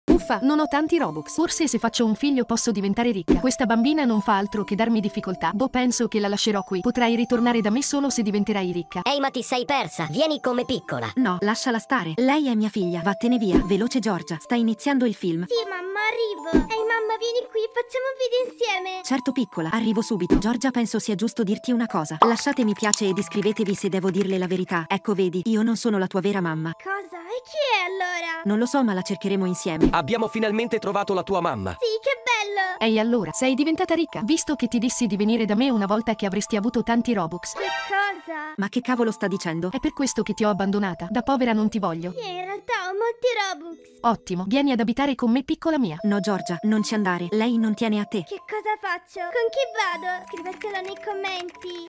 LA FIGLIA HA RITORVATO SUA MAMMA MA NON è COME SE L'ASPETTAVA!!
Ciao a tutti amoriə! 👑💜

♡ IGGì 📸 LaPrincipessaGiorgia